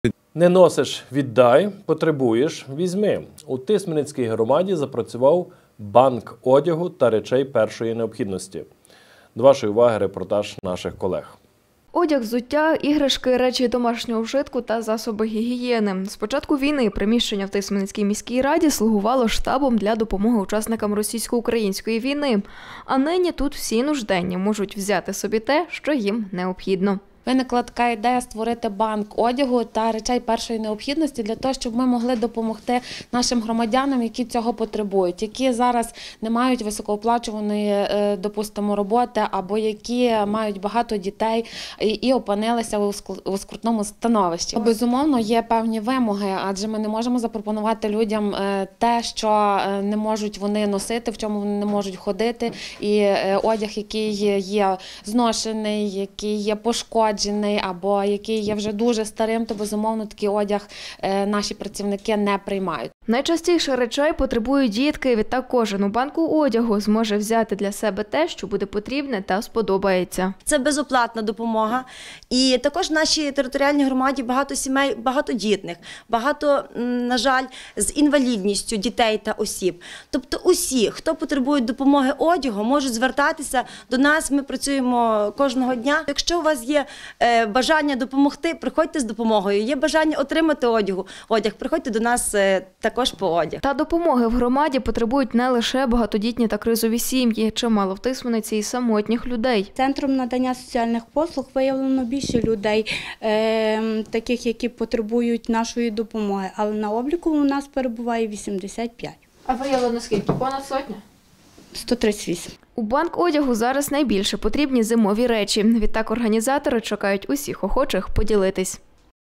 Не носиш - віддай, потребуєш - візьми!
Не носиш - віддай, потребуєш - візьми! У Тисменицькій громаді запрацював «Банк одягу та речей першої необхідності».